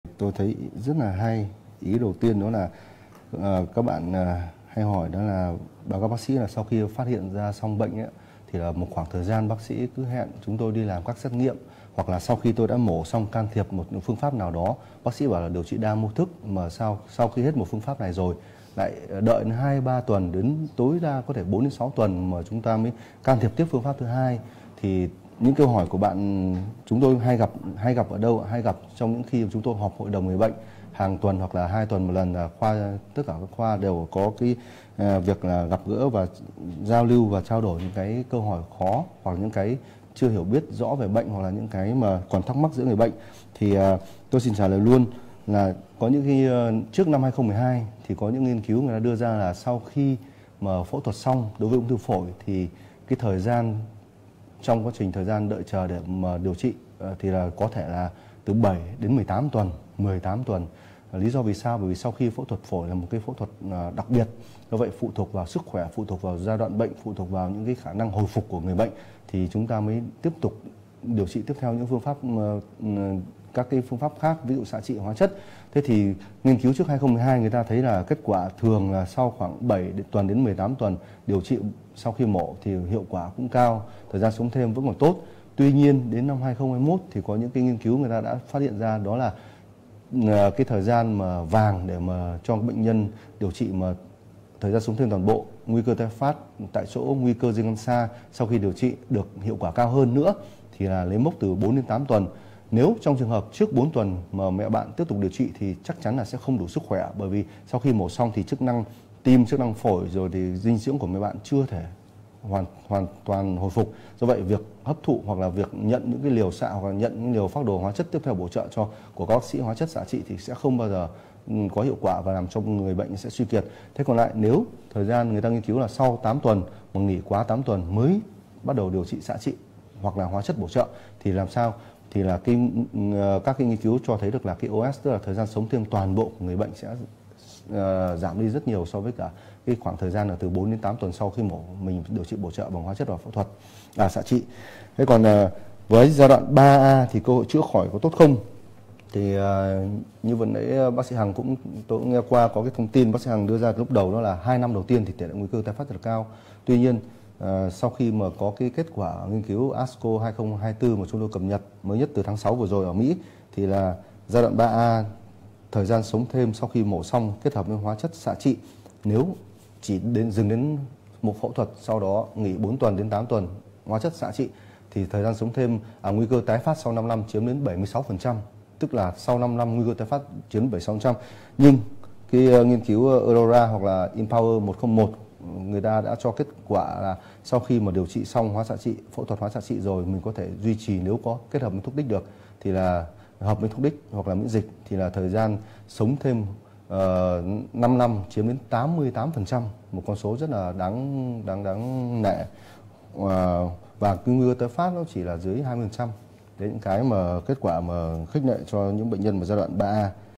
Vì sao sau mổ cần 1 khoảng thời gian mới hóa xạ trị, như vậy thì có nguy cơ di căn không?
Trường hợp người bệnh ung thư phổi đã phẫu thuật cắt thùy trên phổi trái, thuộc giai đoạn 3A, hạch xét nghiệm là hạch viêm, có chỉ định 1 tháng sau mổ sẽ hoá xạ trị. Nhiều người bệnh thắc mắc câu hỏi vì sao khoảng thời gian này mới thực hiện hóa xạ trị, như vậy thì có nguy cơ di căn không? Và giai đoạn 3A có nhiều cơ hội không. Bác sĩ sẽ giải đáp ngay ở video dưới đây!